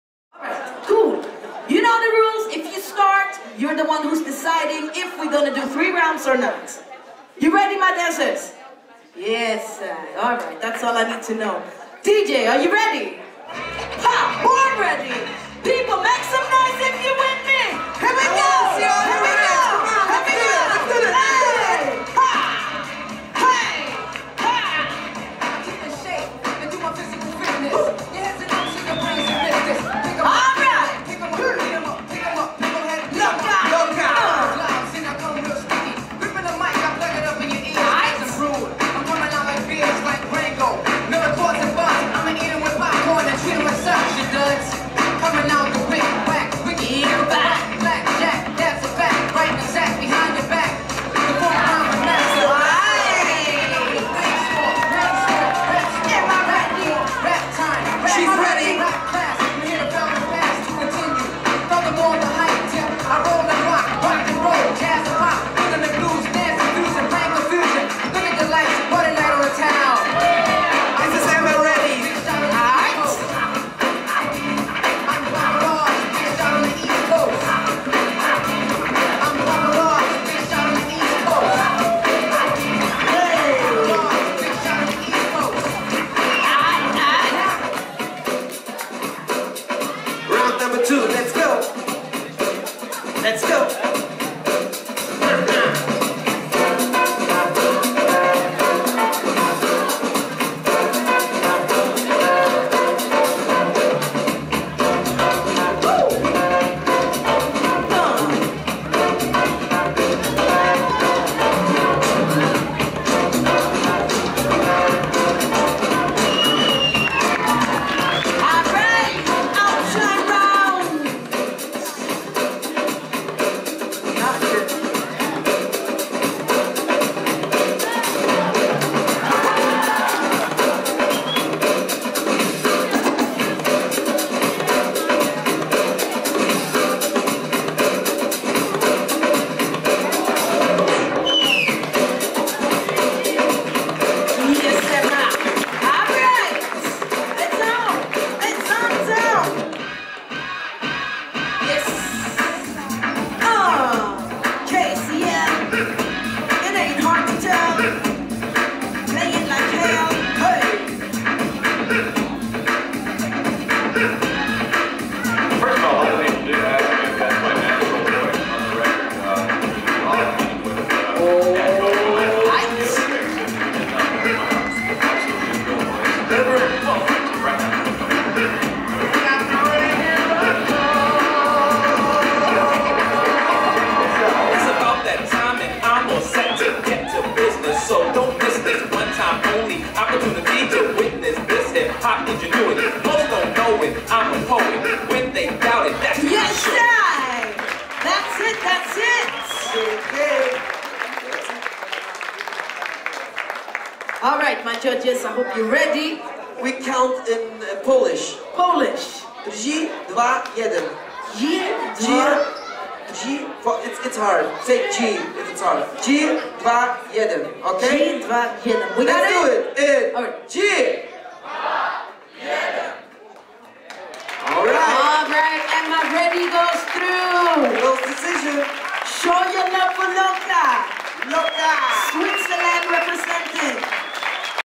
Queen16 Battle 2017 Loca vs. Emma
Top16-Battle of "Queen16"
Sunday, April 30th, 2017
(International B-Girl Contest @ FamilyFever)

Breaking-Festival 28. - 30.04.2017 in Leipzig, Germany.
Top-16-Tournament. Produced by Evil Eve and Loopi.
Video and Editing by Andreas Grüttner.